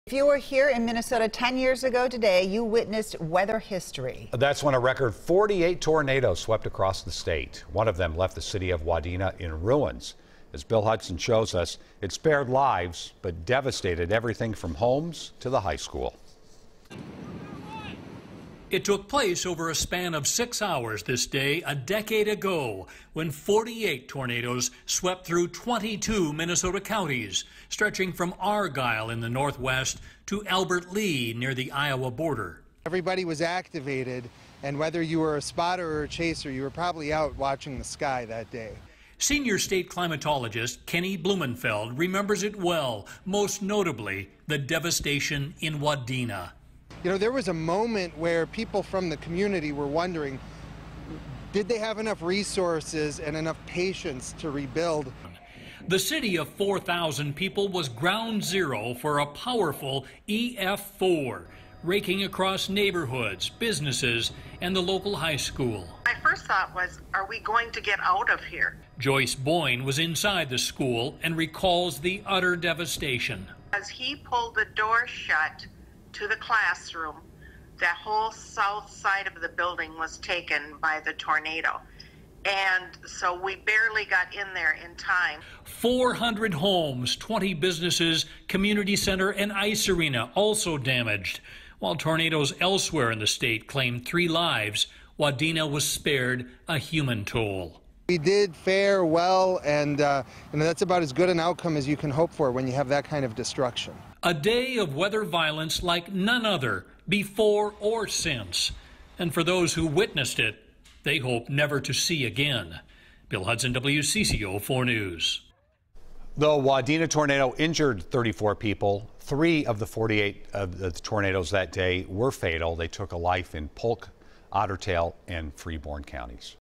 Wadena Looks Back At Day Of Devastating Tornado
If you were here in Minnesota 10 years ago Wednesday, you witnessed weather history, reports Bill Hudson (2:06).